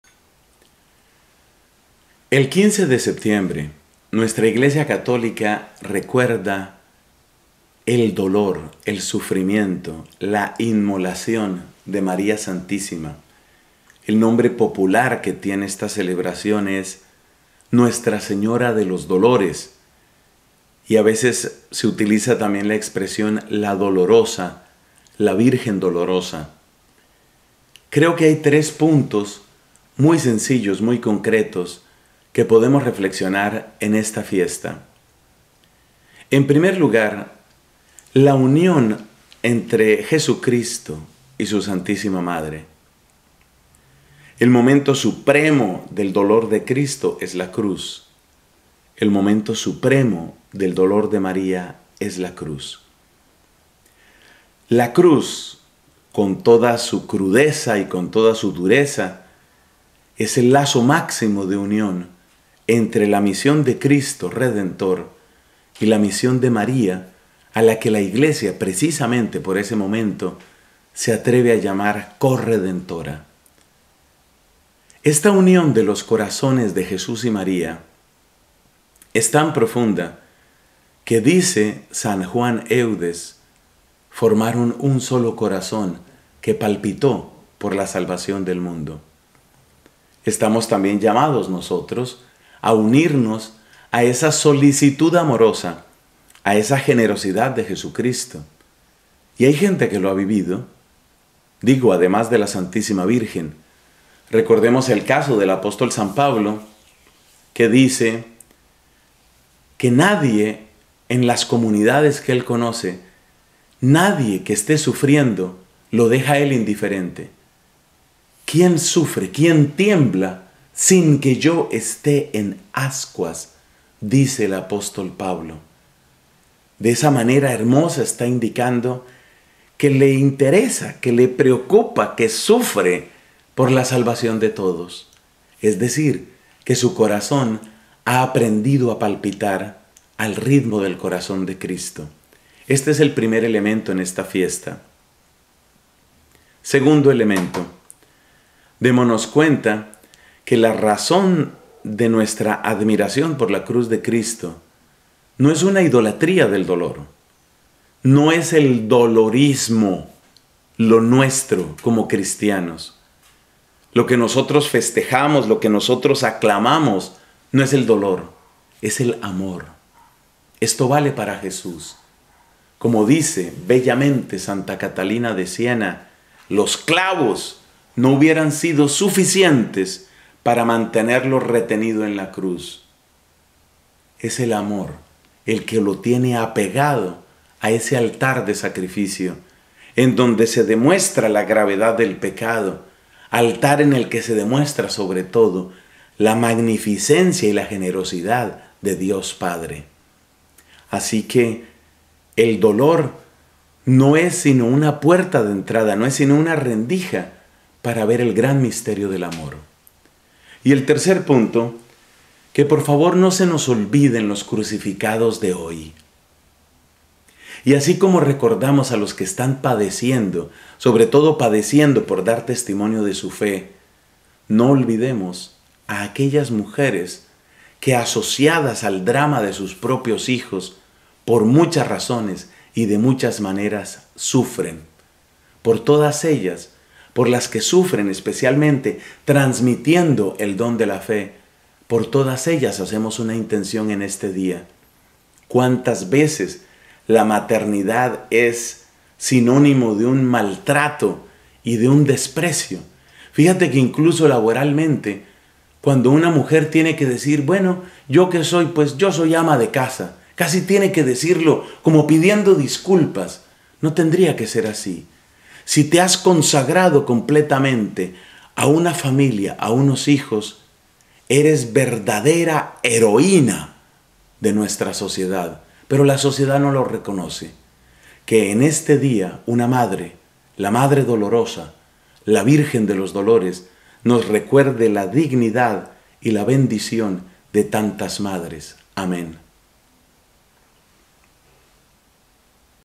LA GRACIA del Lunes 15 de Septiembre de 2014
MEMORIA DE NUESTRA SEÑORA DE LOS DOLORES

El dolor de la Virgen es expresión de la fusión de su amor con el amor de su Hijo, redentor del mundo.